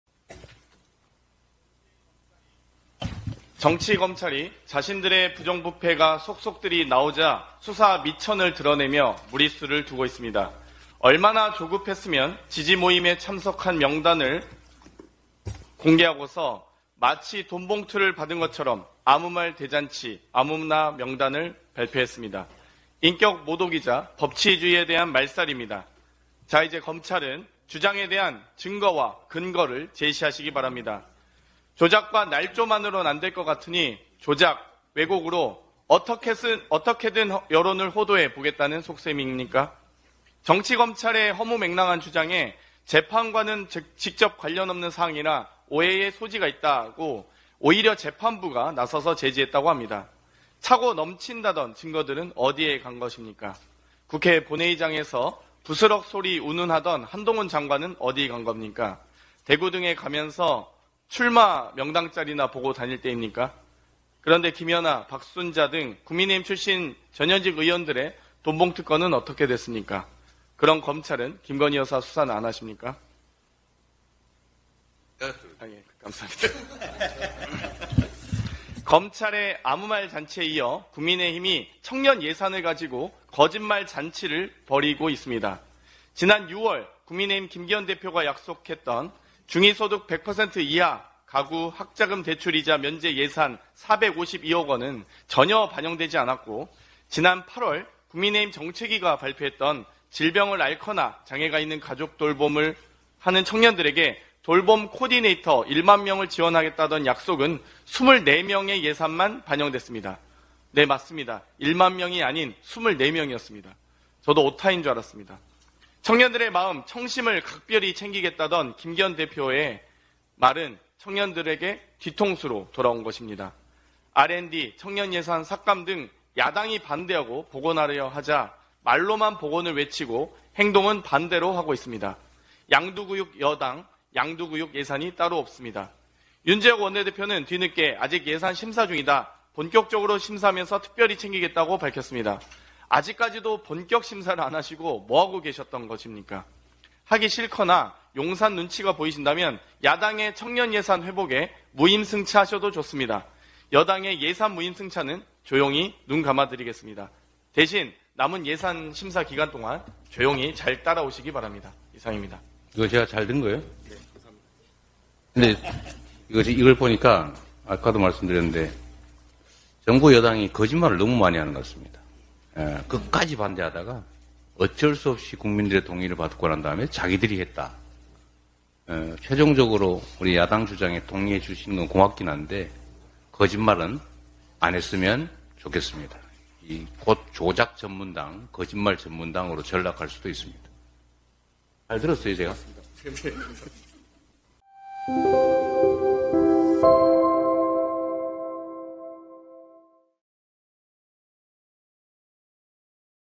장경태, 뒷통수 맞은 청년예산 성토에 이재명 “윤정권 거짓말 너무 많이 해”
2023년 11월 22일 국회에서
더불어민주당 최고위원회의가 진행되었다.